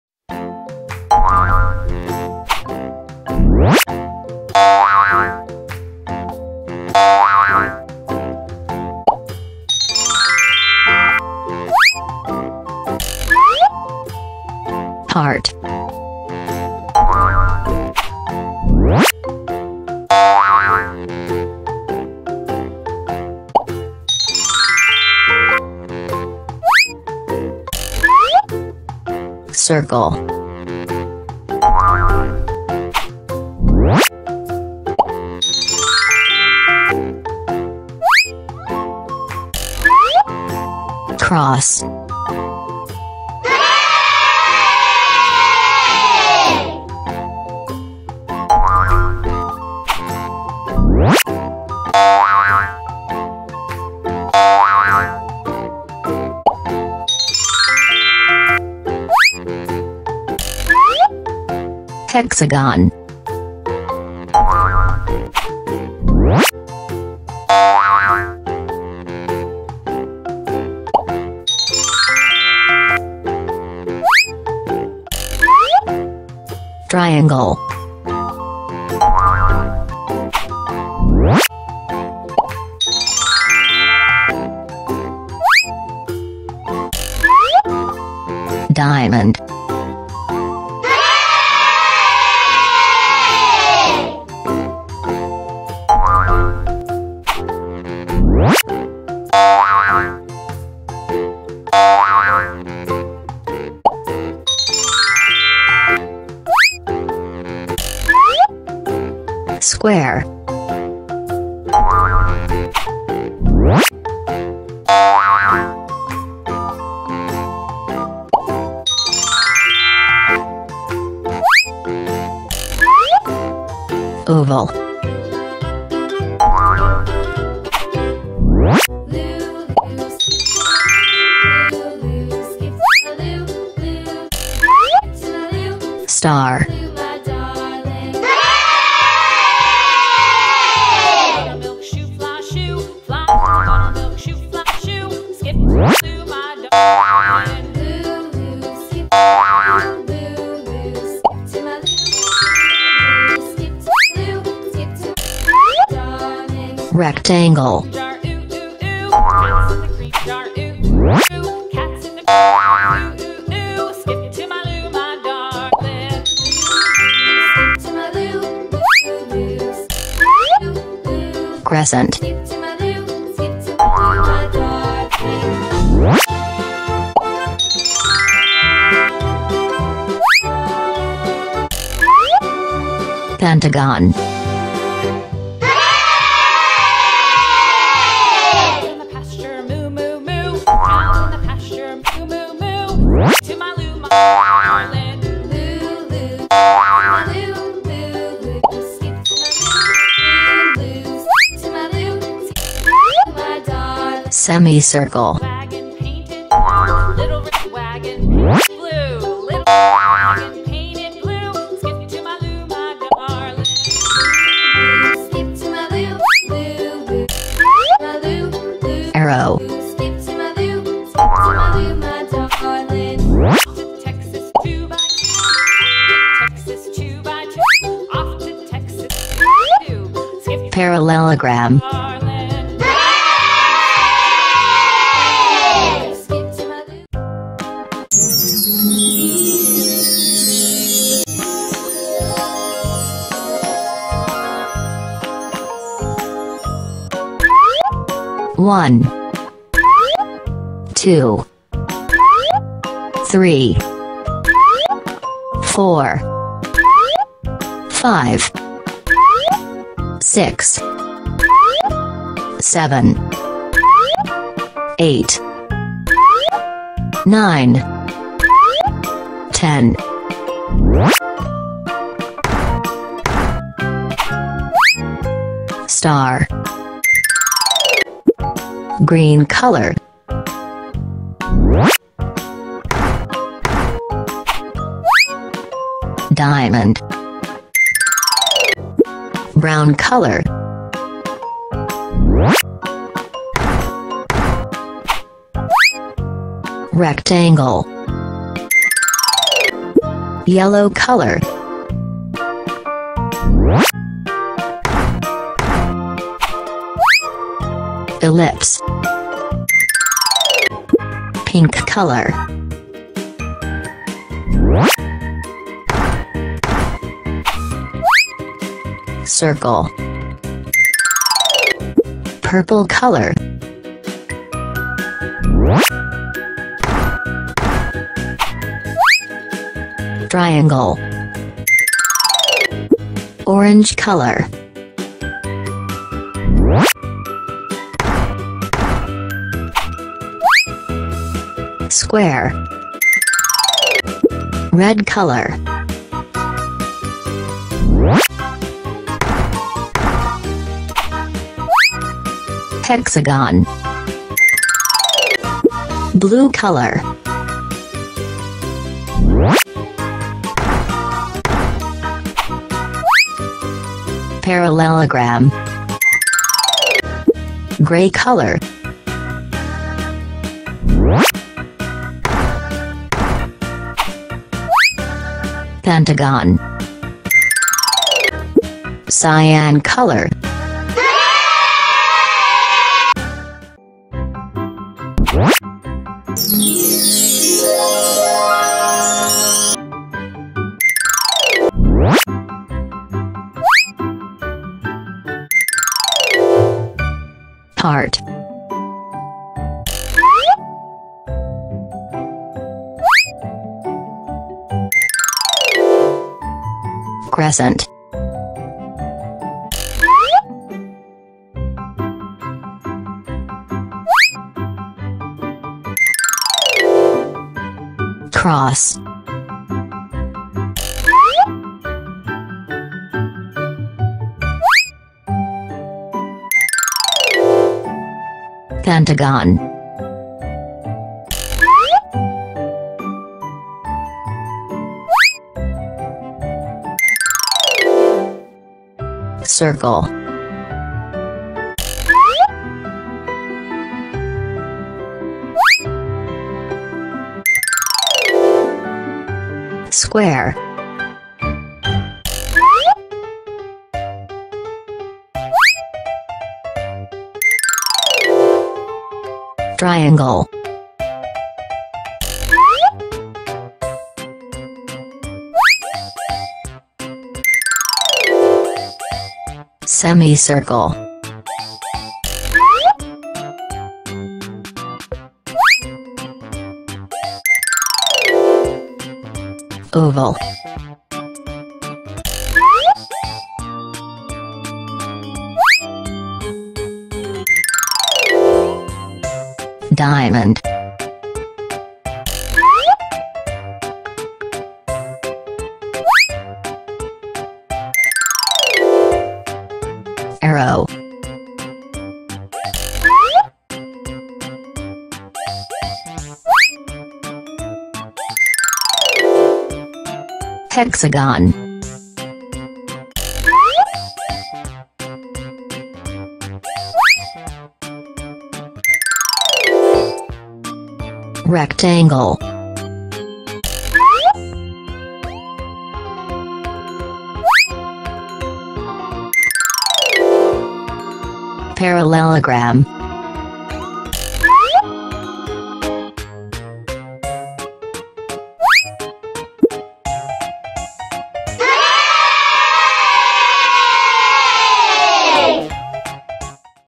Learning Shapes With Wooden Shapes Puzzle | Shapes Puzzle For Kids Preschoolers | ANIMATION ZONE
Kids,toddlers can learn shapes with this educational video. Learn shapes for children. This video is for education of kindergarten.

- Animation Zone